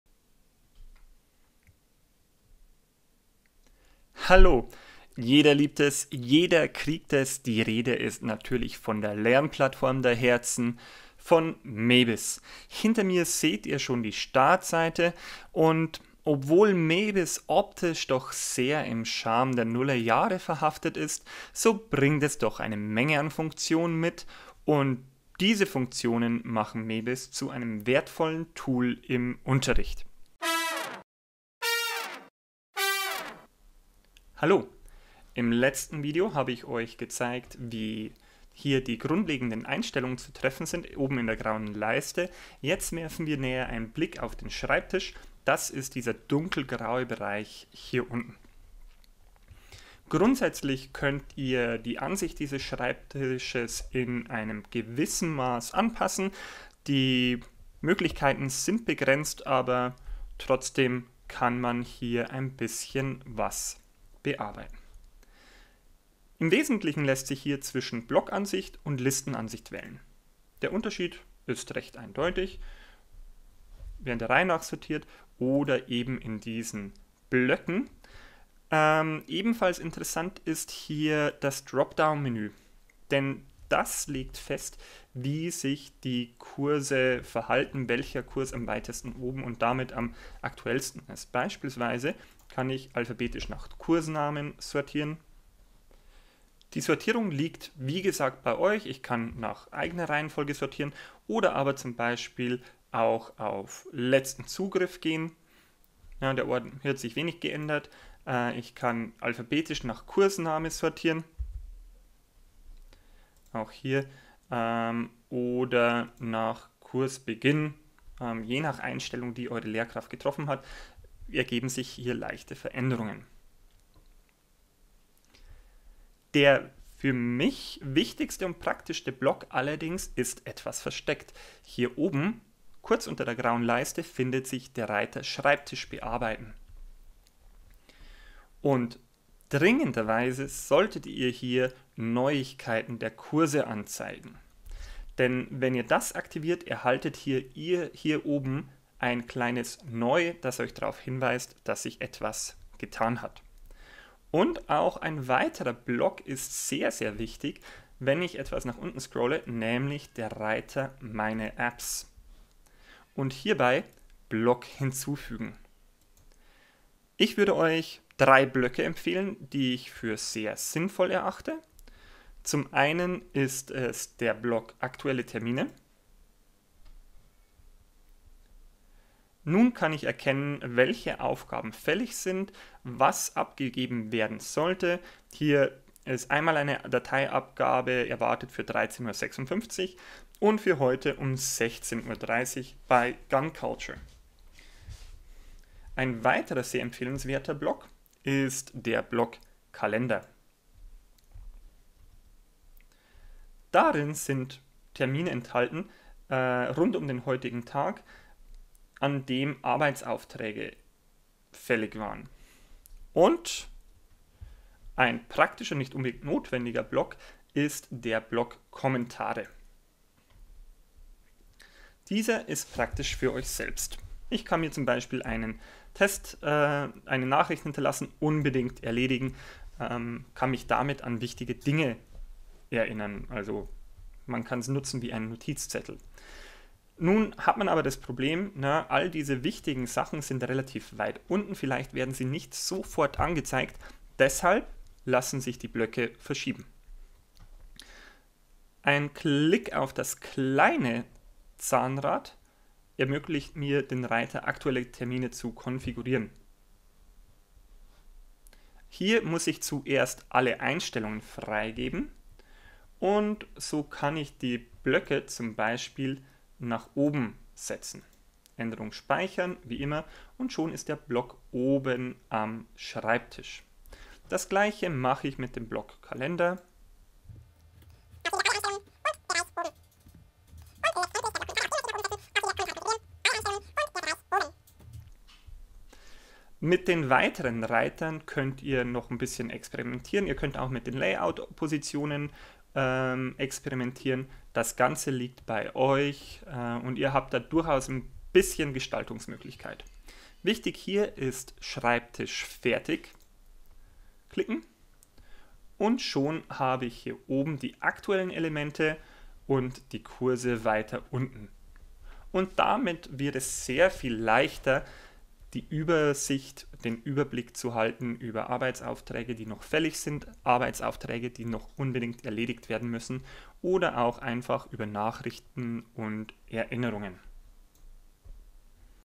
Mebis für Schüler Teil2: Der Schreibtisch
Mit einem ordentlichen Schreibtisch behältst du auch bei der Arbeit mit Mebis den Überblick.